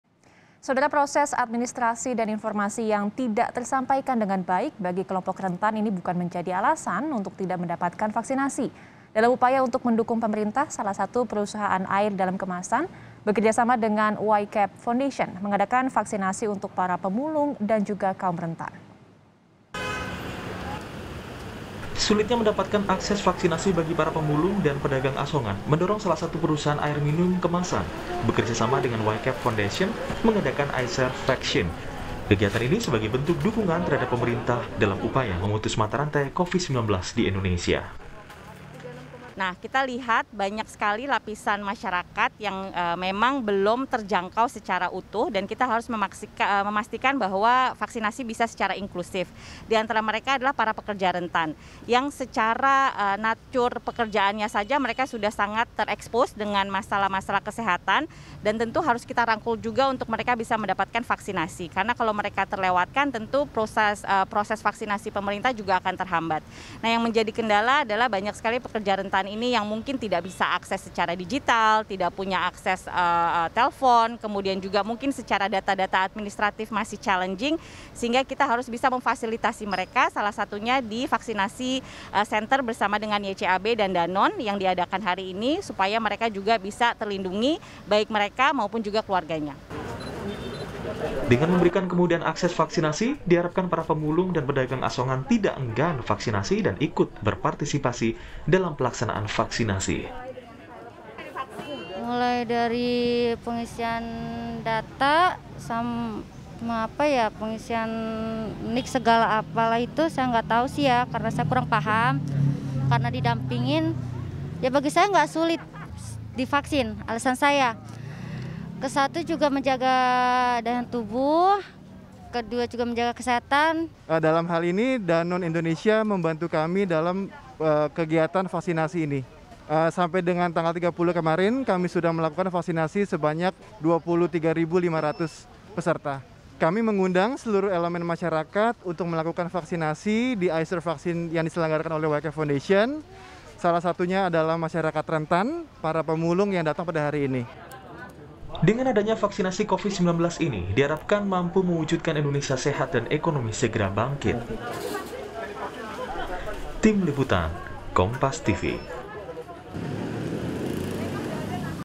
Bersama Peduli Vaksin untuk Kelompok Rentan
JAKARTA, KOMPAS.TV - Proses administrasi dan informasi yang tidak tersampaikan dengan baik bagi kelompok rentan bukan menjadi alasan untuk tidak mendapatkan vaksinasi.

Dalam upaya untuk mendukung pemerintah, salah satu perusahaan air dalam kemasan bekerja sama dengan YCAB Foundation mengadakan vaksinasi untuk para pemulung dan kaum rentan.

Kegiatan ini sebagai bentuk dukungan terhadap pemerintah dalam upaya memutus mata rantai Covid-19 di Indonesia.

Dengan memberikan kemudahan akses vaksinasi diharapkan para pemulung dan pedagang asongan tidak enggan vaksinasi dan ikut berpartisipasi dalam pelaksanaan vaksin.

"Dalam hal ini Danone Indonesia membantu kami dalam hal vaksinasi ini, sampai dengan tanggal 30, kami sudah melakukan vaksinasi sebanyak 23.500 peserta, kami mengundang seluruh elemen masyarakat untuk vaksinasi di i-serve vaccine yang di selenggarakan oleh YCAB Fondation. Salah satunya adalah masyarakat rentan para pemulung yang datang pada hari ini," kata Ketua Panitia Vaksin YCAB Foundation, James Revelino.

Diharapkan dengan adanya vaksinasi ini mampu mewujudkan indonesia sehat dan ekonomi segera bangkit.